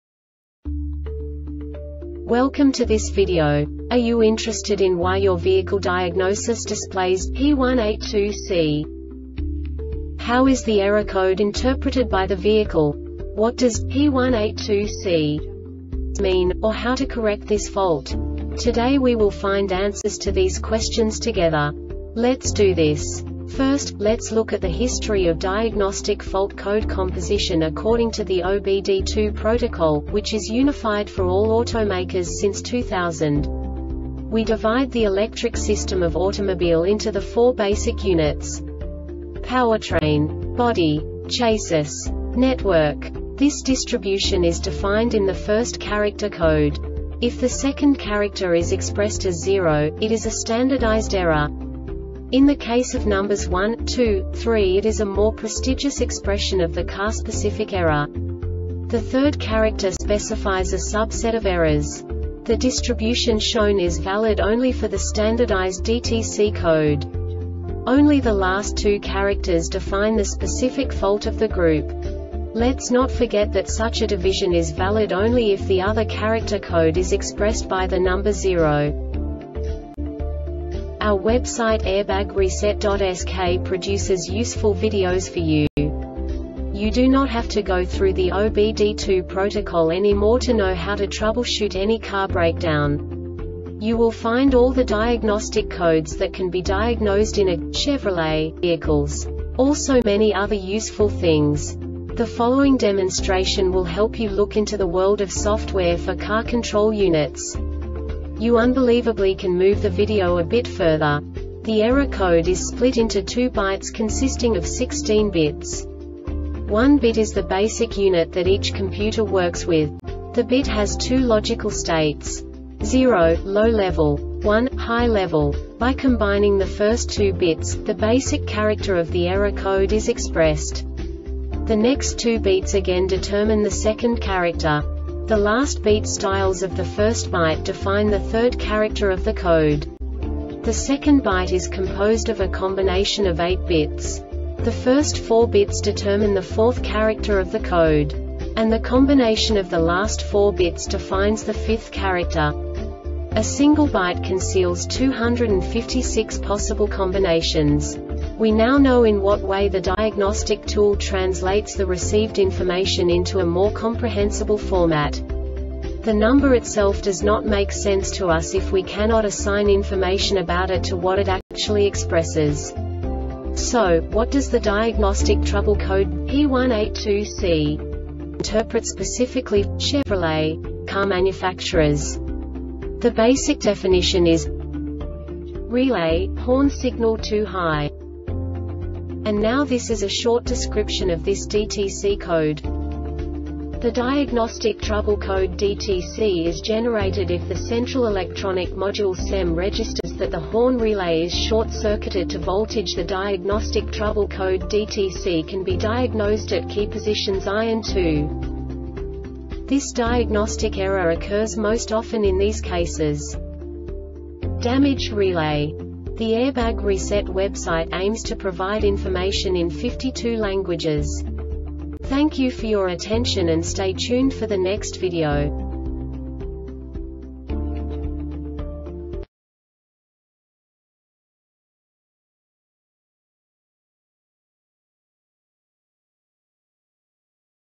DTC Chevrolet P182C Short Explanation
Contents:
0:21 Basic DTC analysis according to OBD2 protocol standard.
1:48 Insight into programming
2:58 A diagnostically understandable description of the Diagnostic Trouble Code
3:05 Basic error definition

Make:
Chevrolet
Code:
P182C
Definition:
Relay, Horn Signal Too High
Description:
The diagnostic trouble code (DTC) is generated if the Central Electronic Module (CEM) registers that the horn relay is short-circuited to voltage.The diagnostic trouble code (DTC) can be diagnosed at key positions I and II.
Cause:
Damaged relay.